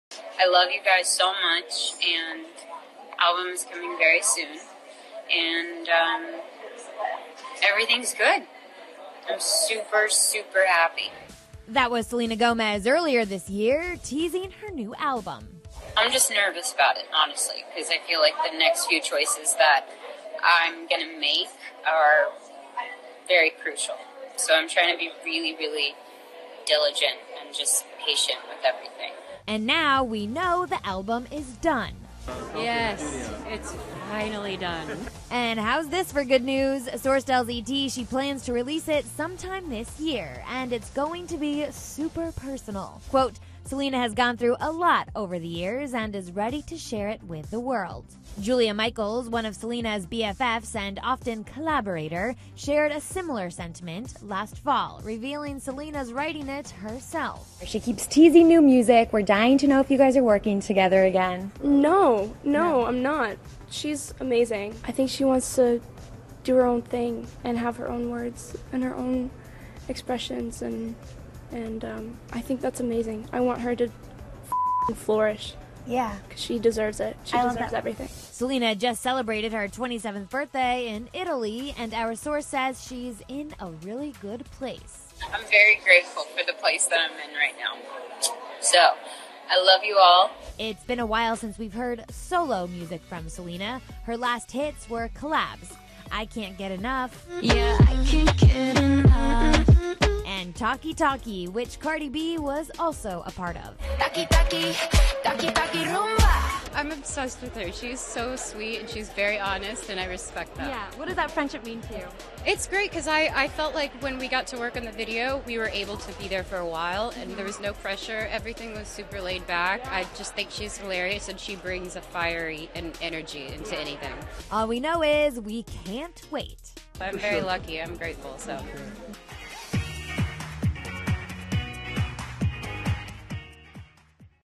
Selena Gomez's New Music Will Be 'Very Personal' (Source)
ET has learned that Selena Gomez is ready to share her 'personal' new music with the world!